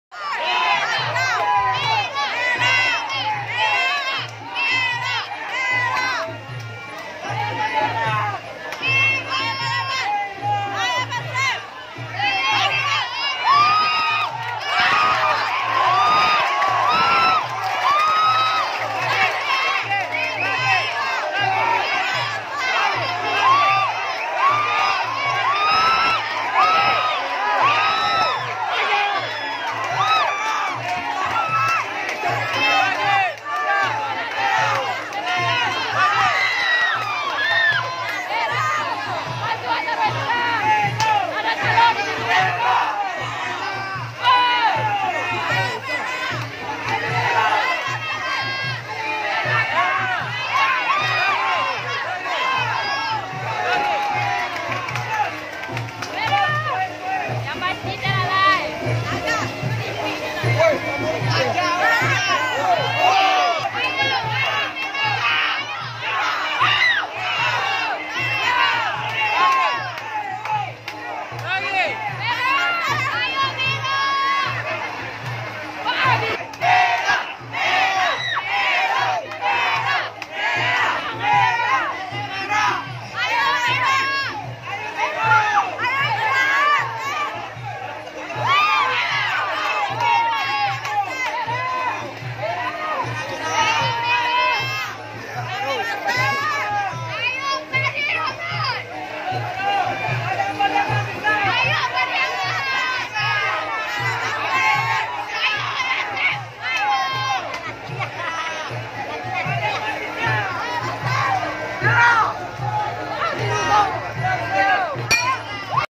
Babak ke 3 Yosep Tia Juwisean (Red) Vs Rada Irman (Blue) cabor Muaythai Porprov Sumbar 2018
Publik boleh menilai, siapa pemenangnya berbeda dengan keputusan juri